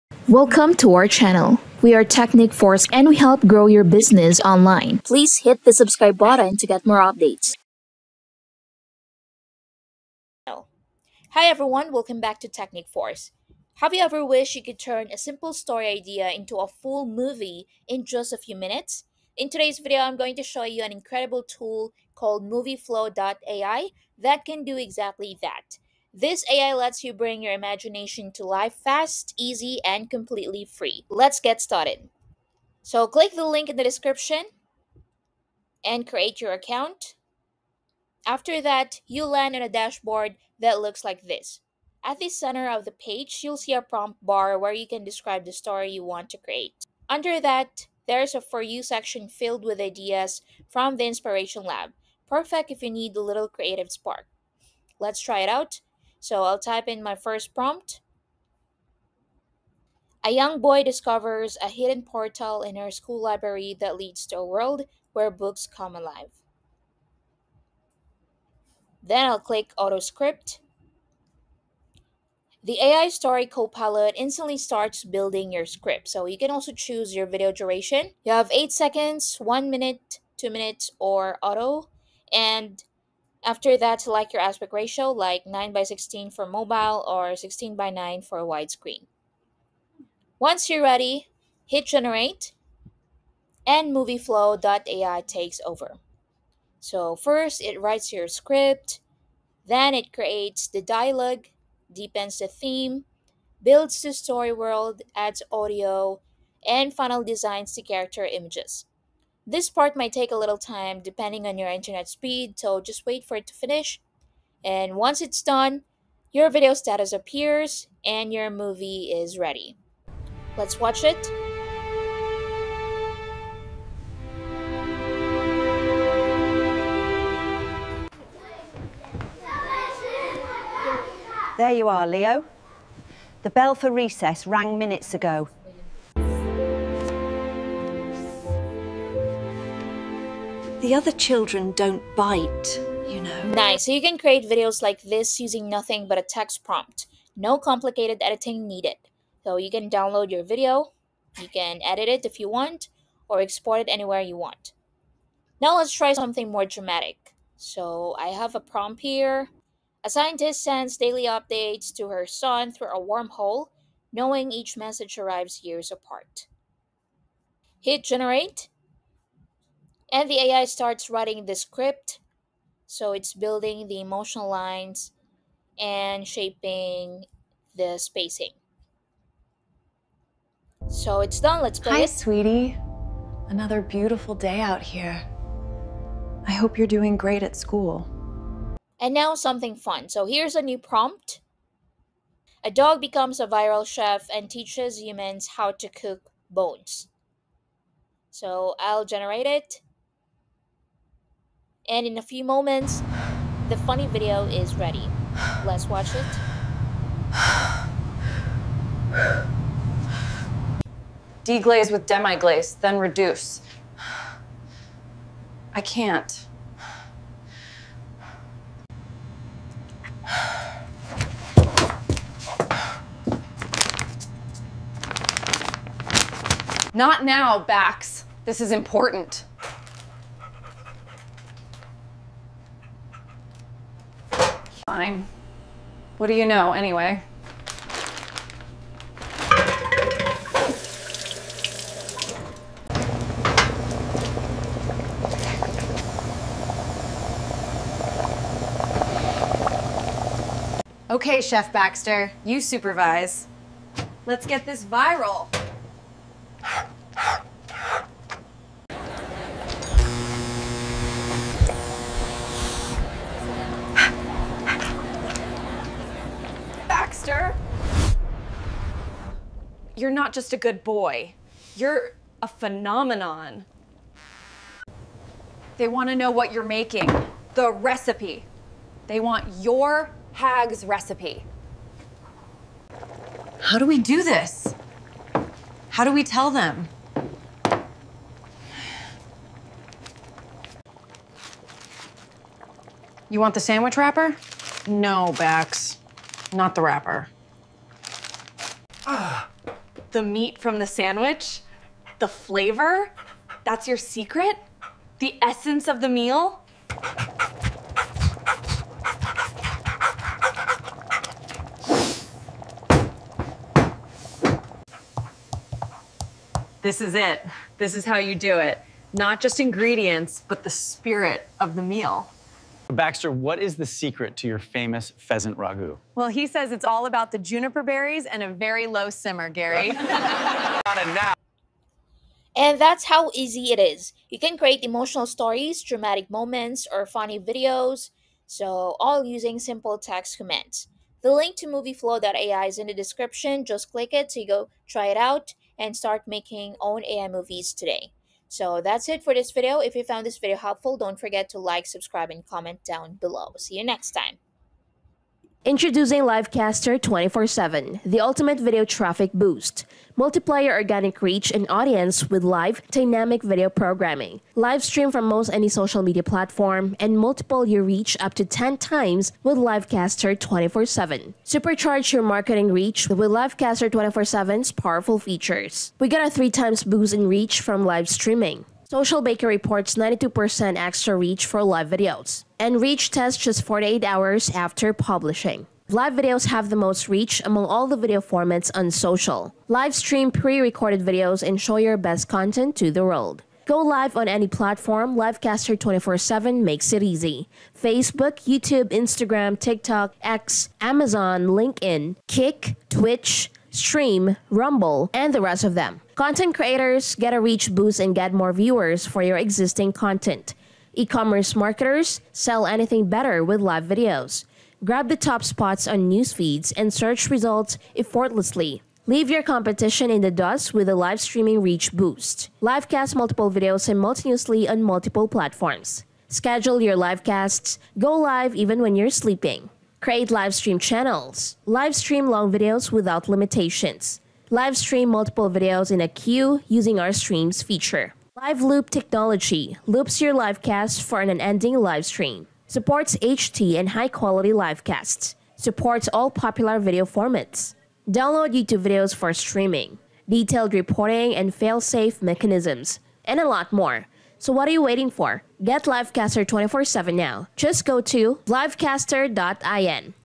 How to Create Long YouTube (16:9) Videos Using MovieFlow AI | FREE & UNLIMITED
Want to turn a simple story idea into a full movie in just minutes? In today’s video, I’m revealing an incredible tool called MovieFlow.ai — an AI platform that transforms your imagination into cinematic scenes fast, easy, and 100% free. Whether you’re a creator, storyteller, or someone who just wants to experiment with AI filmmaking, this tool will blow your mind. I’ll walk you through exactly how it works and how you can start creating your own mini-movies in no time. Let's dive in!

Also Watch:

🔥 Livecaster24×7
The Ultimate Video Traffic Boost